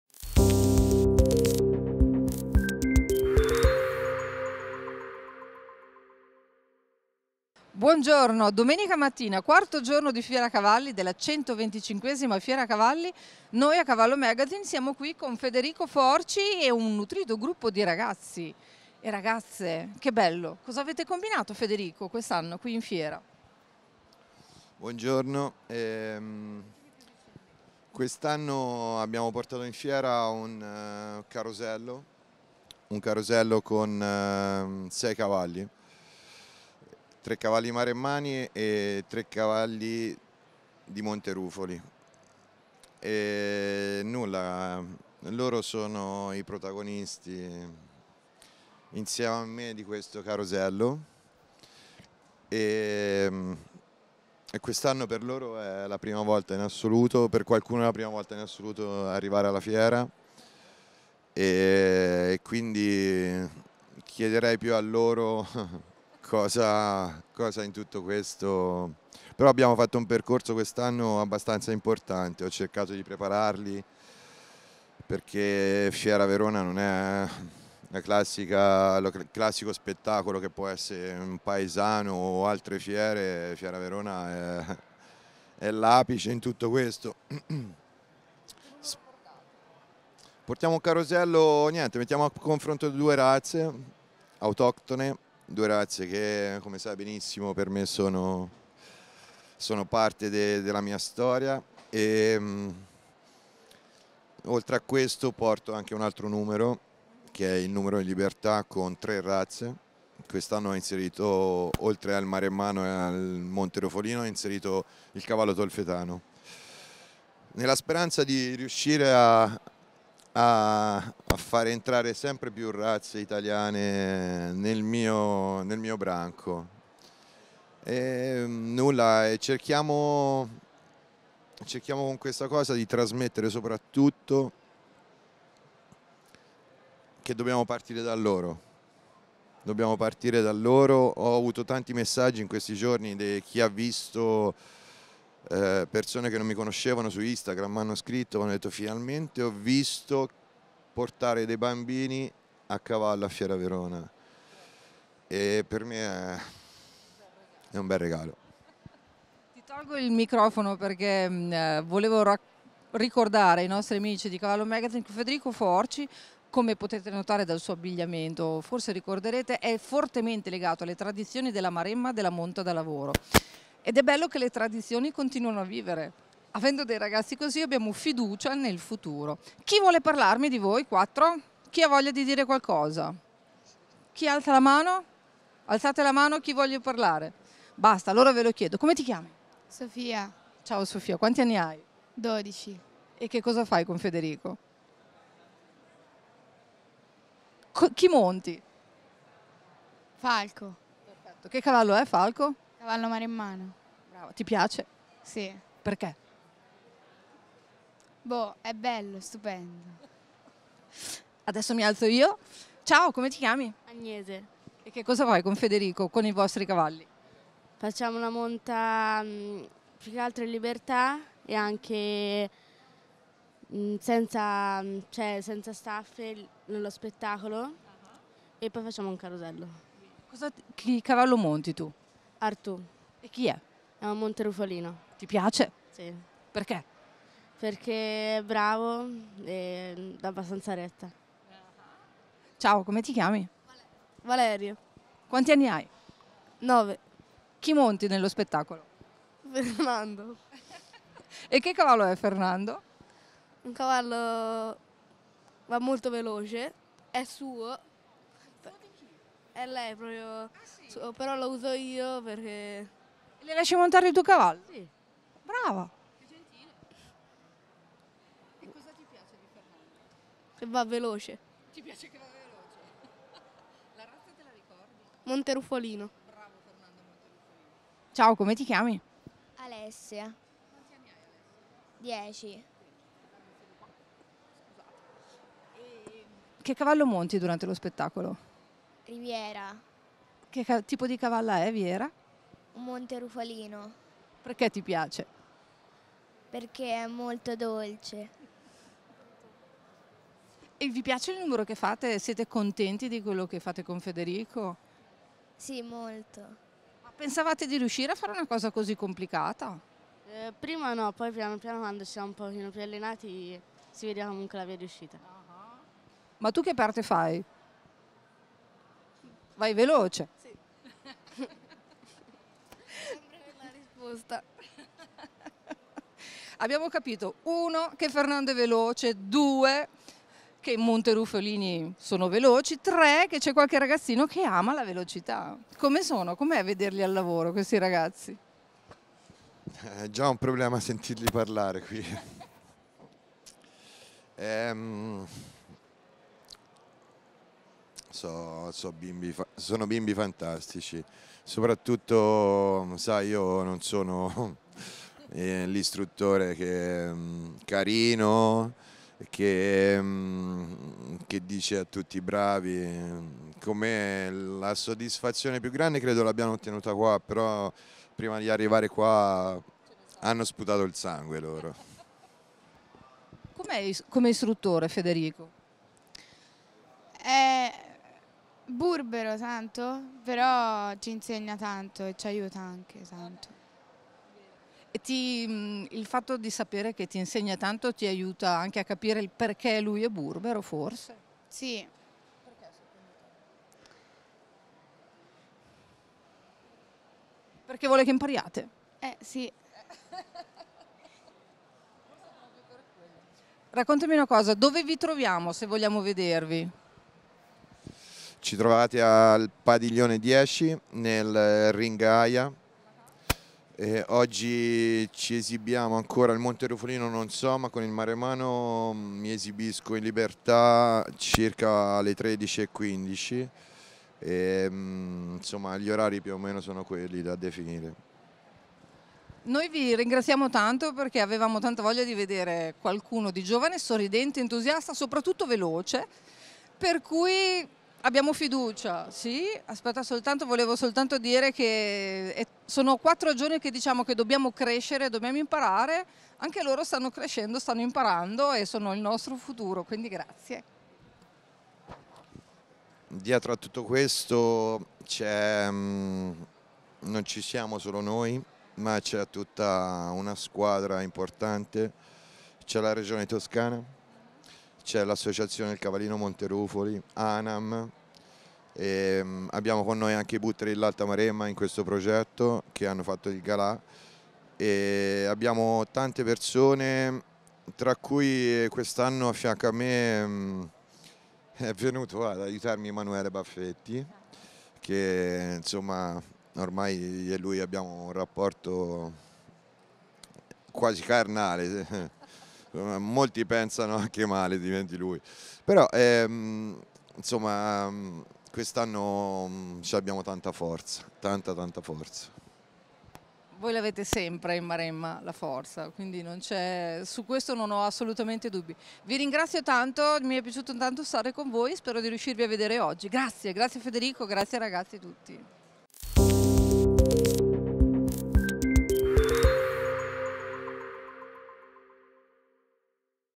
Fieracavalli Verona 2023 - Il Carosello di Federico Forci
Giorno 4 - Domenica 12 novmebre
Un po' di Carosello... con Federico Forci e i suoi ragazzi!